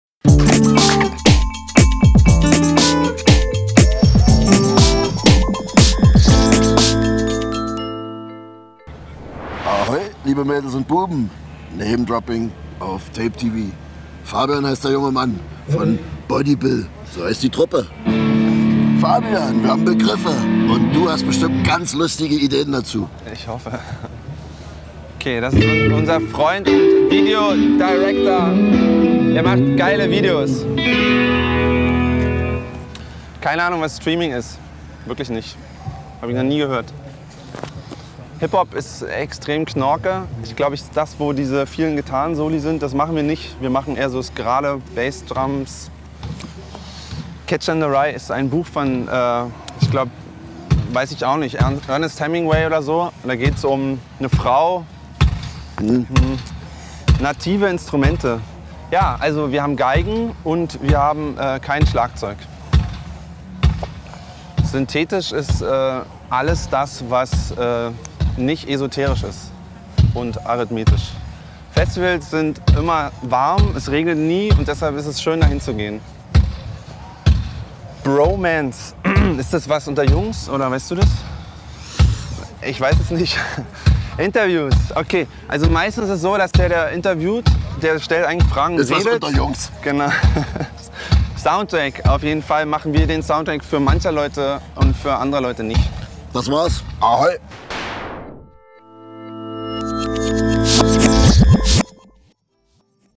Episode #30 | Namedropping | Bodi Bill
Ein Interview im klassischen Sinne ist das nicht, was hier zwischen Rummelsnuff und Bodi Bills Fabian vor sich geht. Das ganze bewegt sich schon eher in Richtung Bromance. eine schöne neue folge Namedropping...

Musikalisches „Dalli Dalli“ to go mit Kapitän Rummelsnuff. Ohne Worte, aber voll auf‘s Maul!